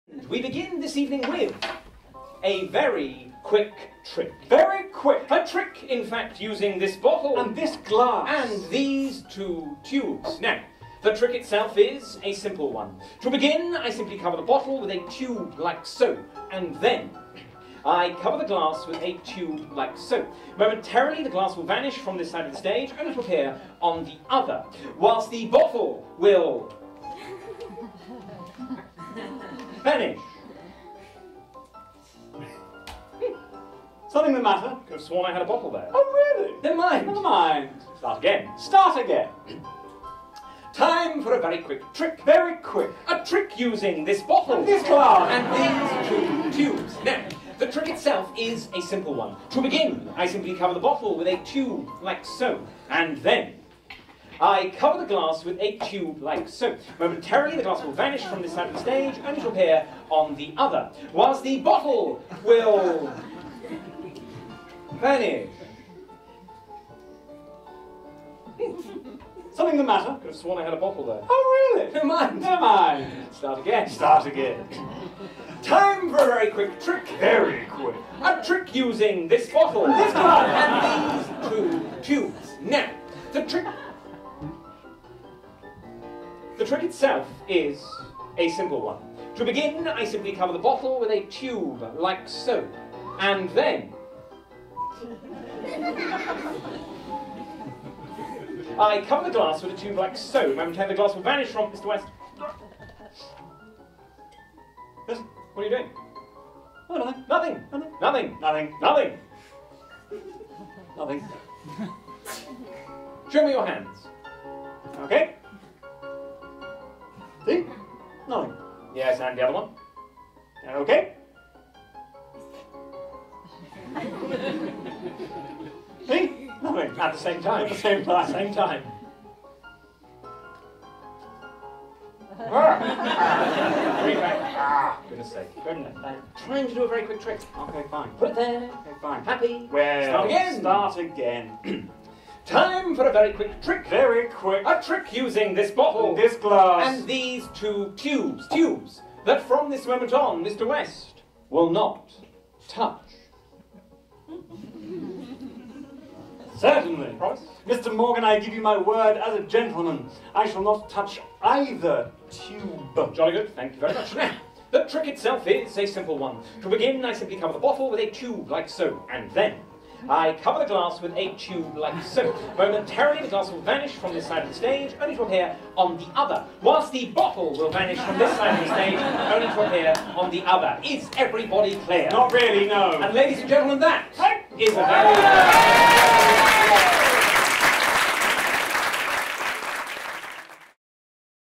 The Restarting Bottle by Morgan & West | Full Performance
Morgan & West perform The Restarting Bottle, a parlour trick. From their book, Parlour Tricks. Which contains, oddly enough, parlour tricks. Available, as if you didn't know, right